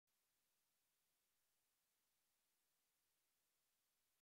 Atmosphere at International Media Corps' 2013 Annual Awar...
Atmosphere at International Media Corps' 2013 Annual Awards Celebration in Los Angeles, CA 11/8/13